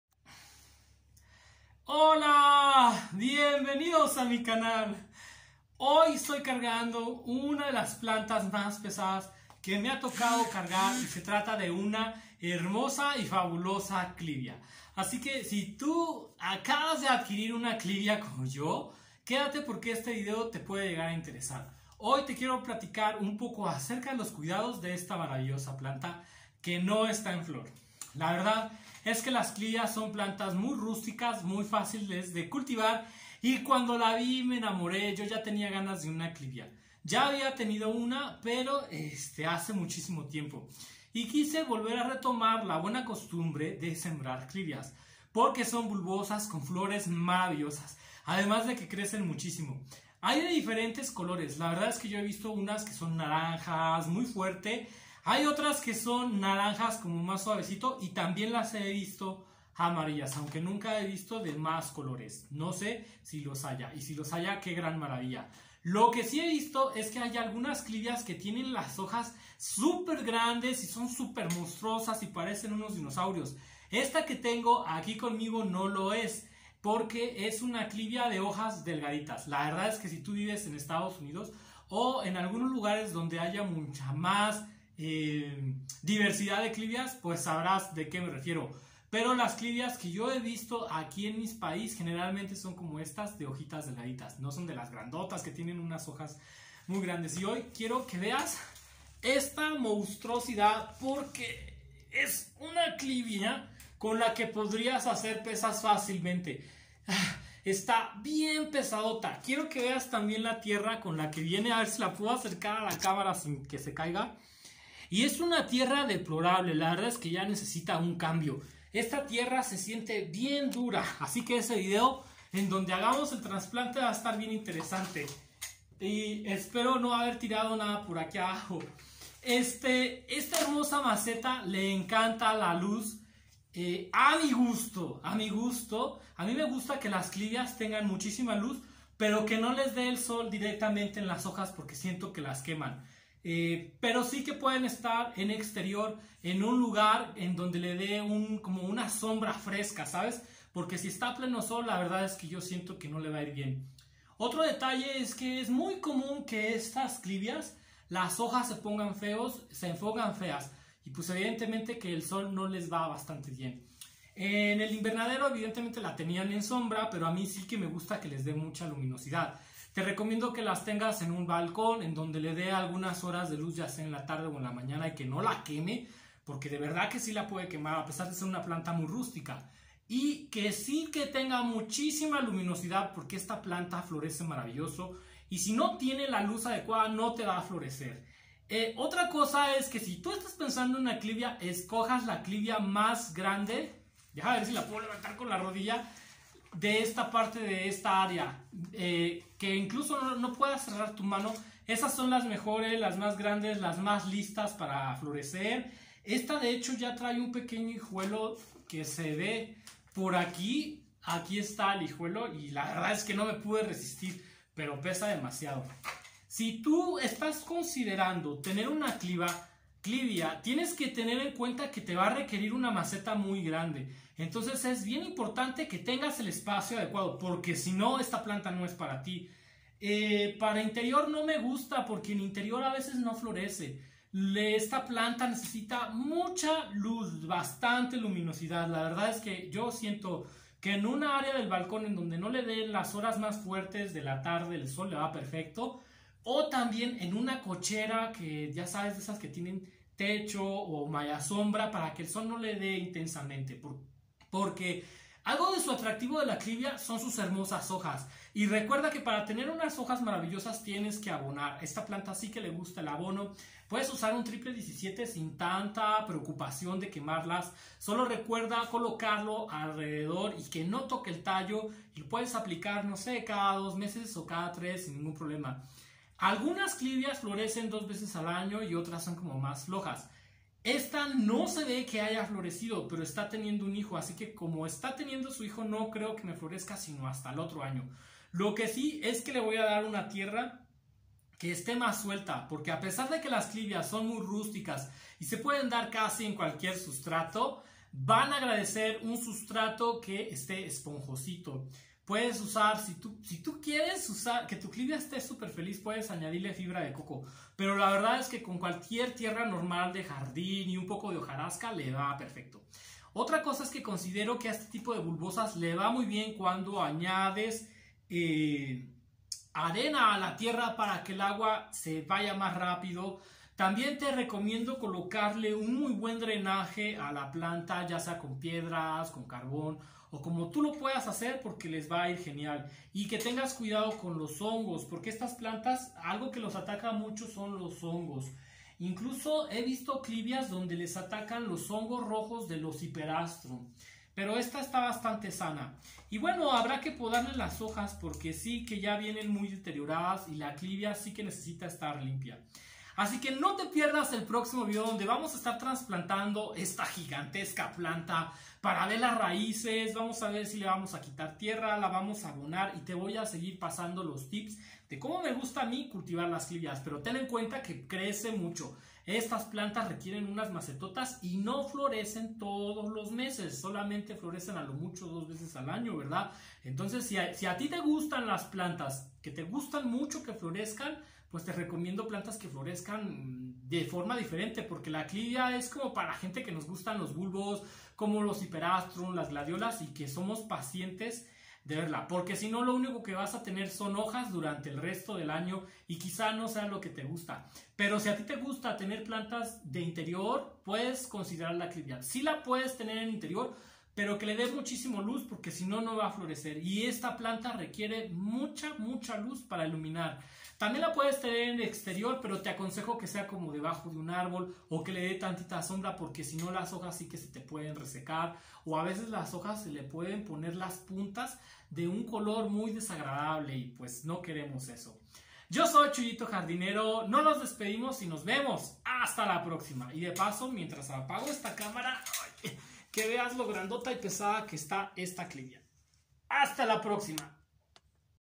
Clivia cuidados y cultivo chuyito jardinero
En este vídeo te cuento como cuido las clivias chuyito jardinero